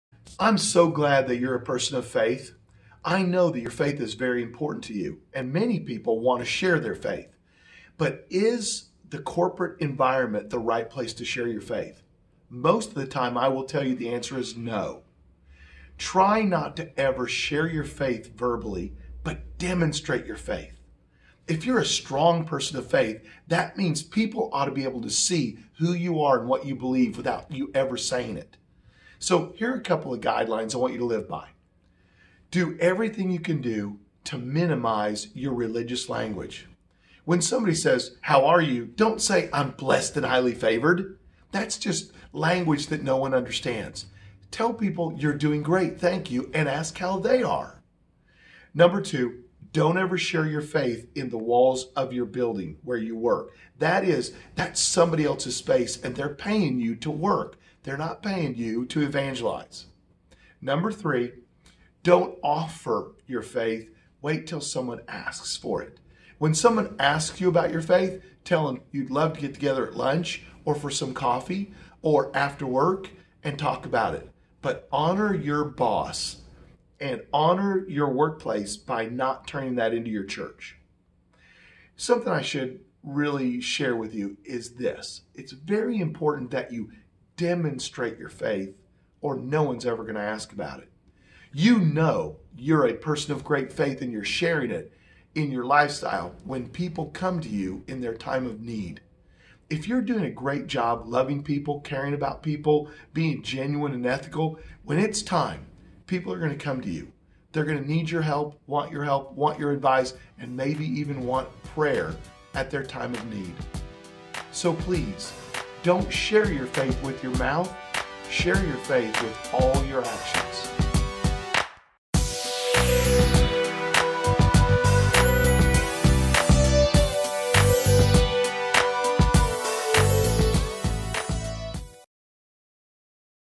Sharing Your Faith at Work
The topic is Evangelizing in the workplace.

Is this okay to do?